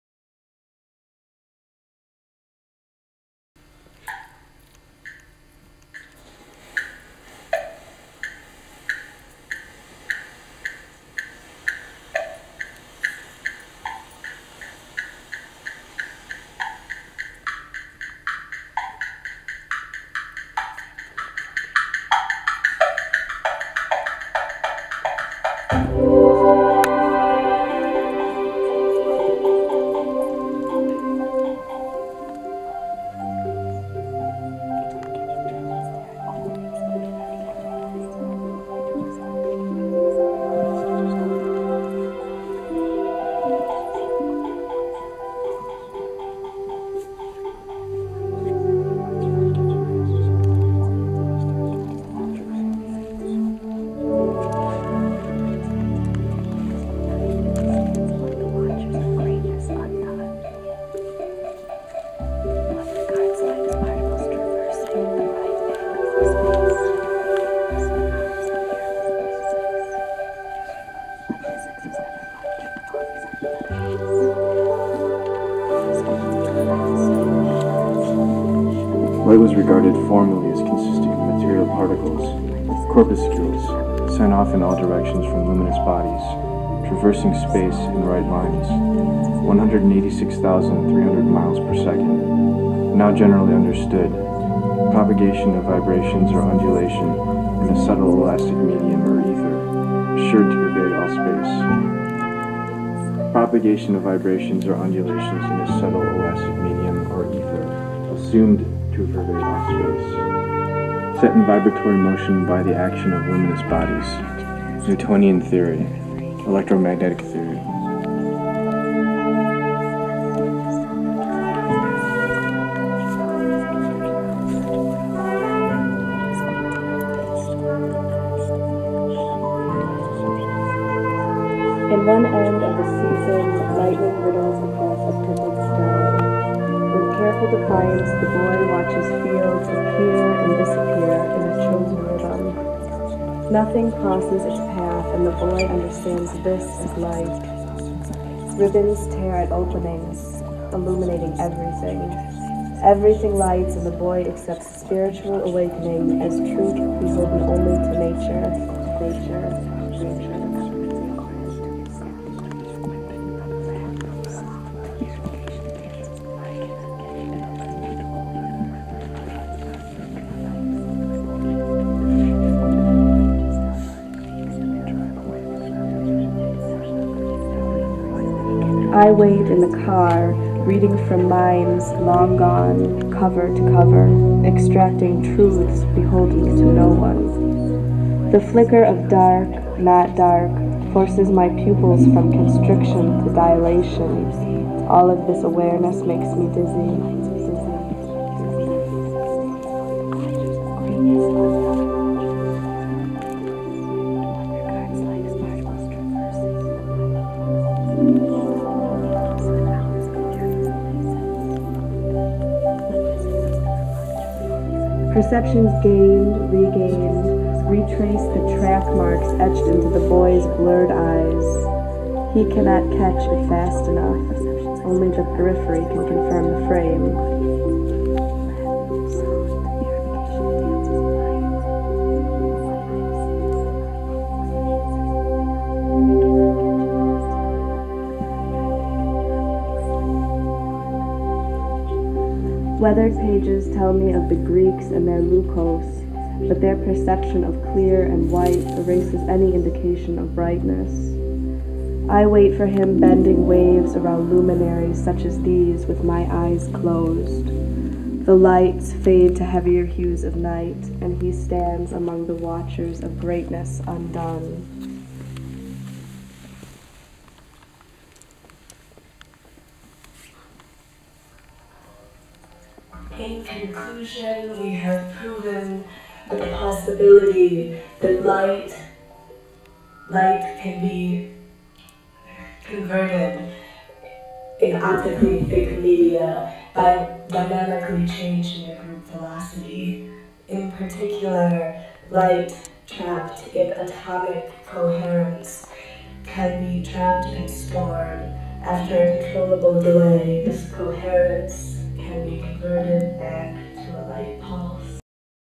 Turning Points 07 Untitled
From a trans-disciplinary class taught in 2001 at the Digital Media Commons (then the Media Union) at the University of Michigan.  Jim Cogswell, Richard Tillinghast, Eric Santos, Tom Bray